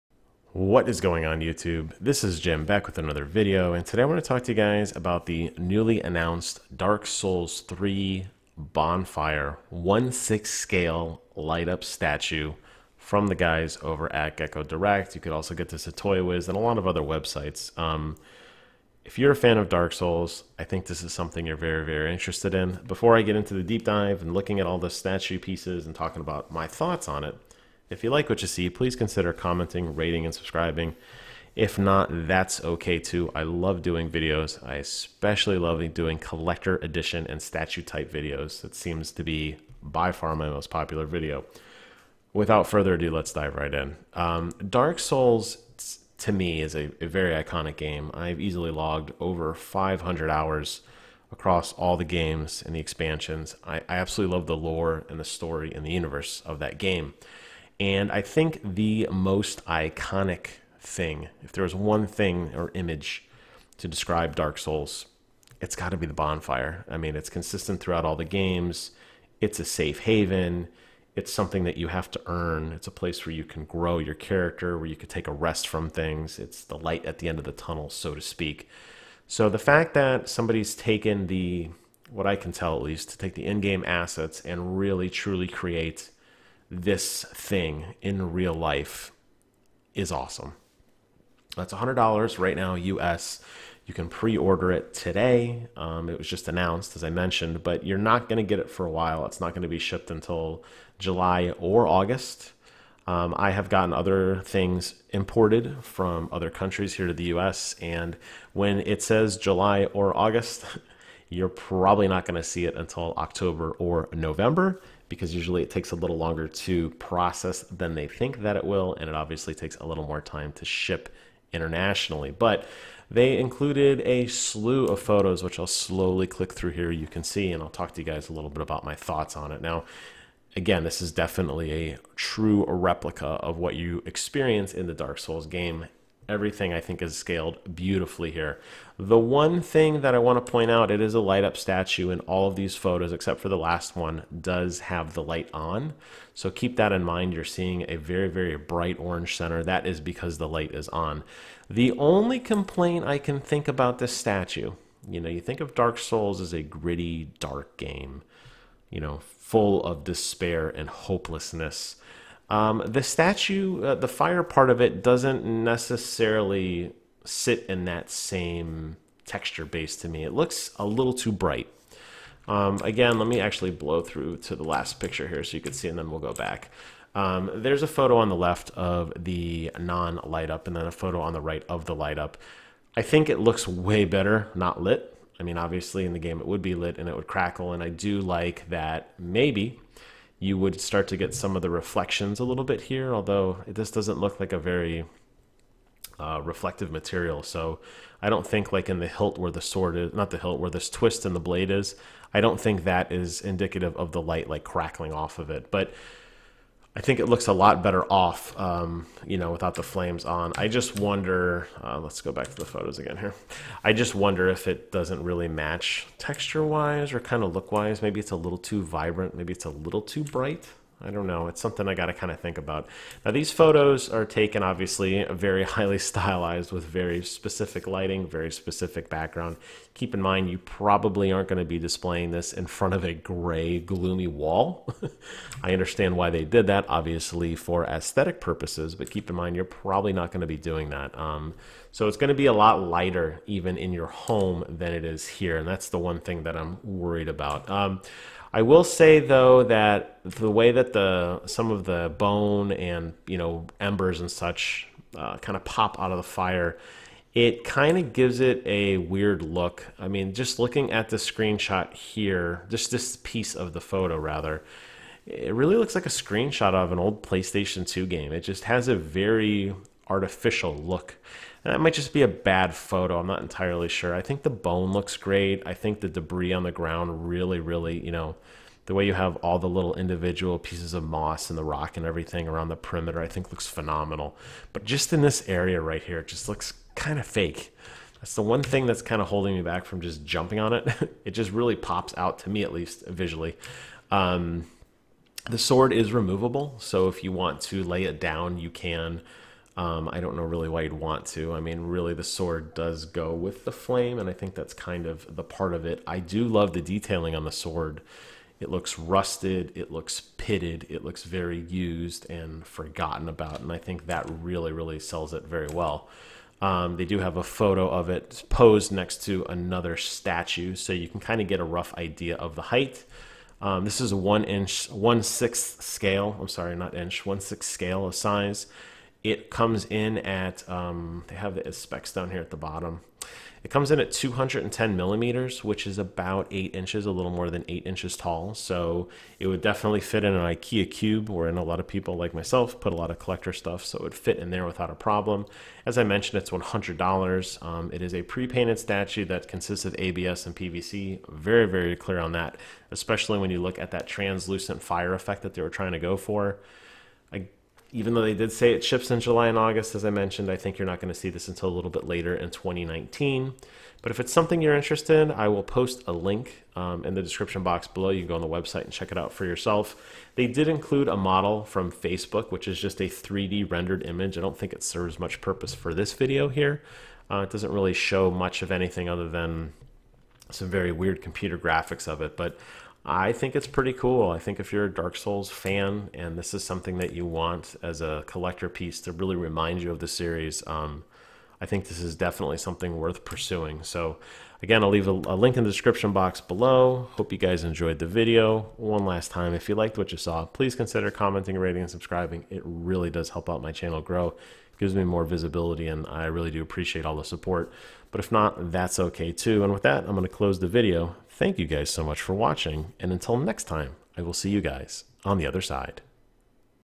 FIRST LOOK!! - Dark Souls 1/6 Scale Bonfire Statue
If you're a fan of Dark Souls, this is something I think you will be very interested in.  It is $99 US and ships later in 2019.

The Bonfire holds many sacred and important meanings in the Dark Souls series. We are excited to present this monumental fire as a 1/6 scale statue.No detail was left uncaptured.

The burnt bones buried in ashes and the coiled sword were traditionally sculpted in clay by Shinya Akao. The complex surging flame was digitally sculpted by YU-KI.

As a surprising feature, the flame is lit by inserting the coiled sword into the earth just like in the video game scene. The details really shine from within, lighted by a special flickering LED.

The bonfire statue displays beautifully as a stand alone piece but also creates an impressive display next to other Dark Souls 1/6 scale statues. The perfect addition to your 1/6 scale Dark Souls statue collection.

Price: US$99.99
Prepainted statue
Material: ABS/PVC
Size:  210mm
Scale: 1/6 Scale
Release: July~August 2019
Sculpt: Shinya Akao (HEADLONG)
Digital Sculpt: YU-KI
Paint: Katsushige Akeyama (-accent-)